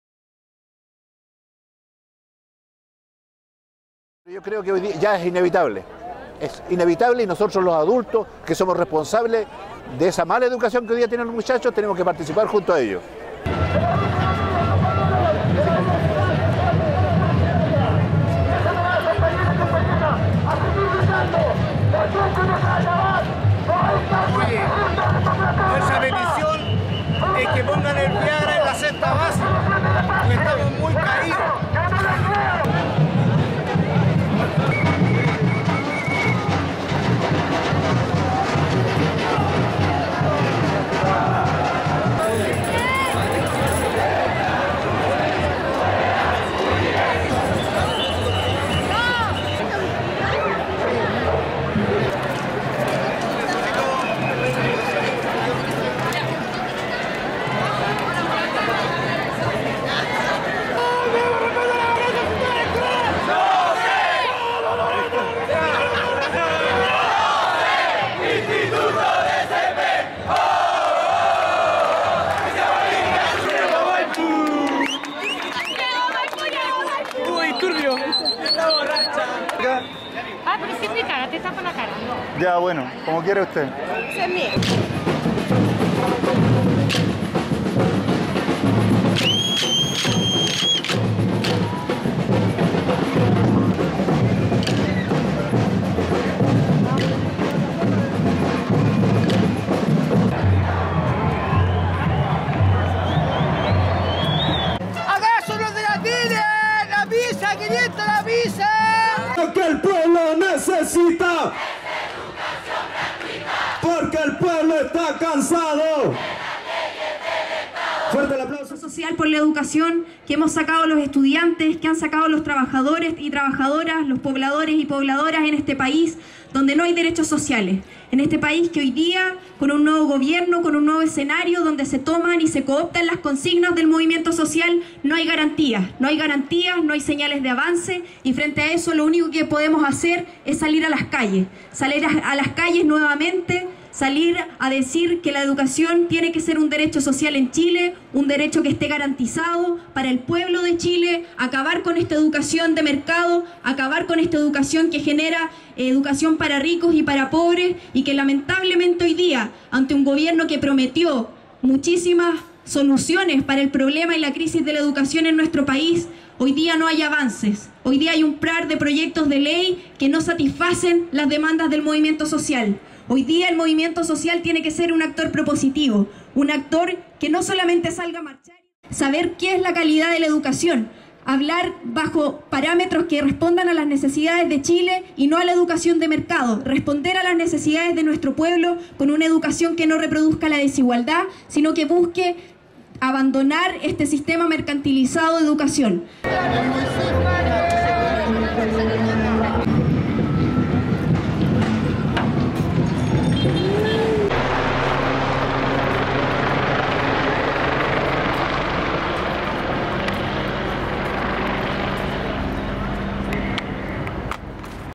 MARCHA POR LA EDUCACION 8 MAYO // 2014 // HARINA TOSTÁ
Marchamos y también creamos el 8 de mayo del 2014.

Una capsula audiovisual de la marcha de l@s estudiantes y de l@s n@ estudiantes, en la primera movilización del 2014 para exigir algo que es normal en cualquier país, menos en Chile: Educación Gratuita y de Calidad.

Nuestra visión: rescatar la creatividad, la alegría, el discurso crítico generado desde el movimiento social.

Te aseguramos que estas imágenes no las verás por la TV.
Contexto: Movilización nacional estudiantil // 8 de mayo 2014. Santiago // Chile.

Edición y Filmación: Hernán Pulgar // Andrés Pinto.

Harina Tostá // 2014.